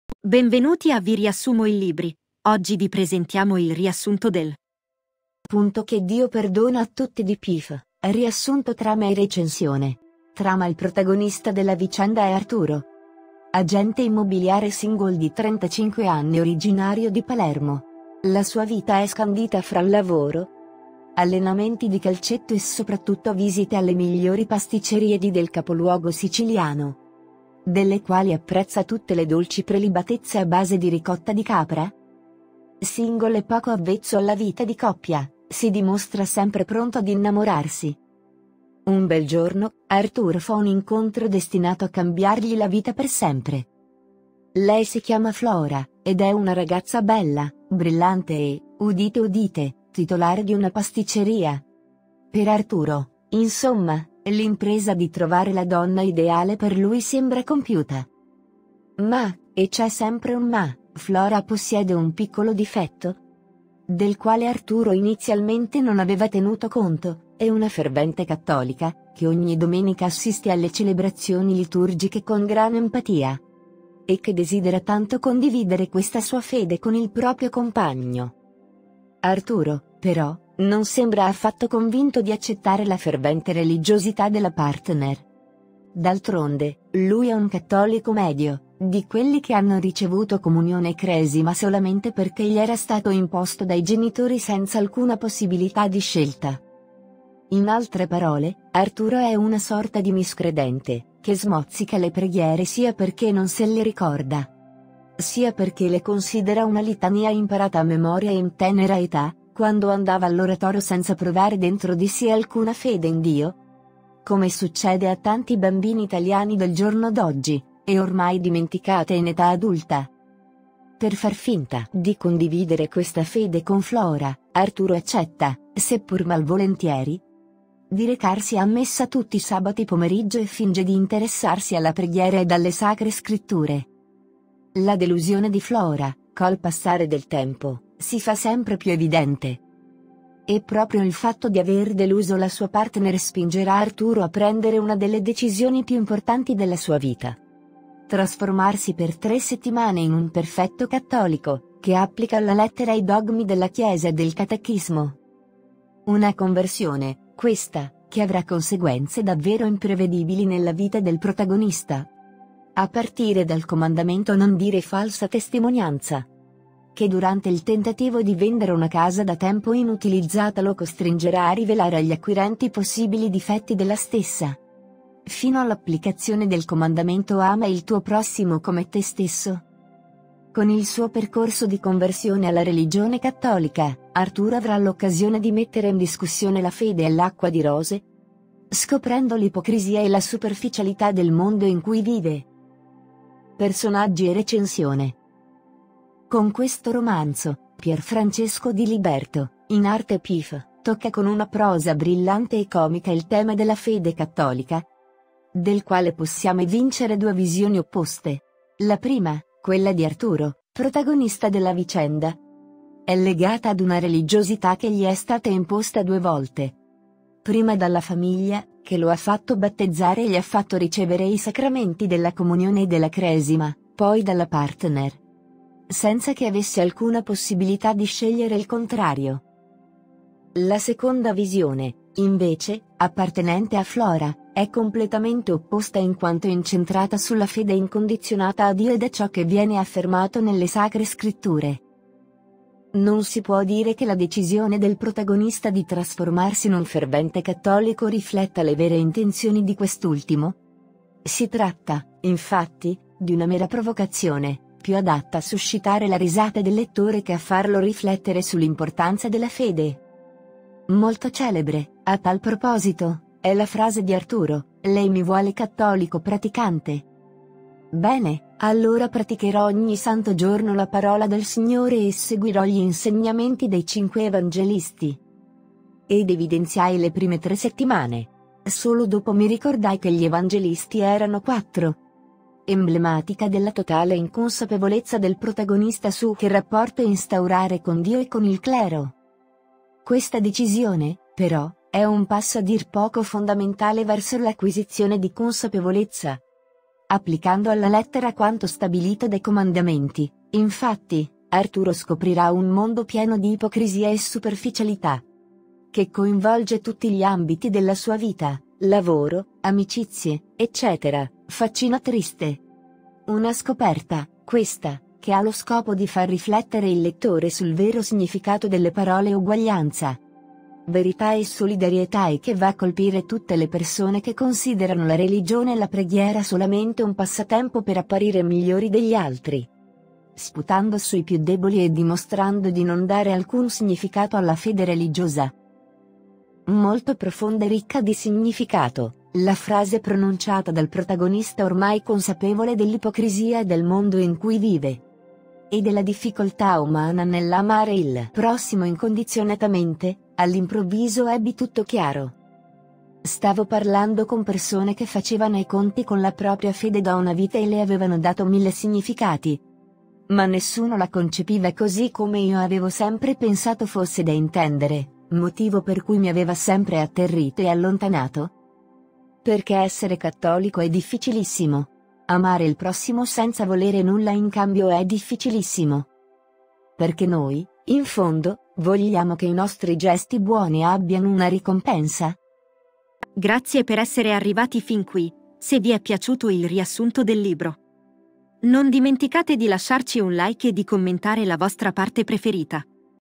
👉🏽 Riassunti …che Dio perdona a tutti di Pif 📖 - TRAMA & RECENSIONE ✅
Riassunto del libri: …che Dio perdona a tutti di Pif ✔️

📖 Oggi vi proponiamo il riassunto del libro: …che Dio perdona a tutti scritto da Pif.

Se vi piacciono i nostri riassunti, lasciateci nei commenti. Quale libro vuole che le riassumiamo?